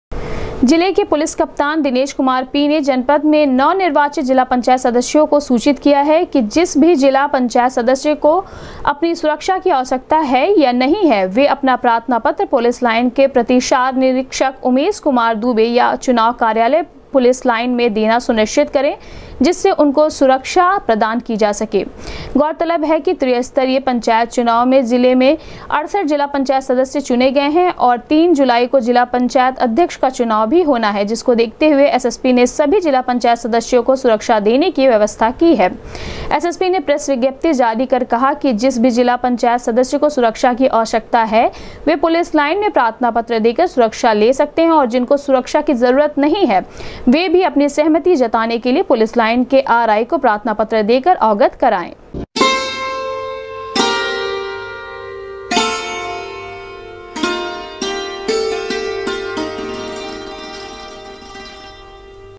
एसएसपी ने प्रेस विज्ञप्ति के माध्यम से दी सूचना,कहा,जिस भी जिला पंचायत सदस्य को सुरक्षा की है
एसएसपी ने प्रेस विज्ञप्ति के माध्यम से दी सूचना,कहा,जिस भी जिला पंचायत सदस्य को सुरक्षा की है आवश्यकता, वह पुलिस लाइन में दे प्रार्थना पत्र, मिलेगी सुरक्षा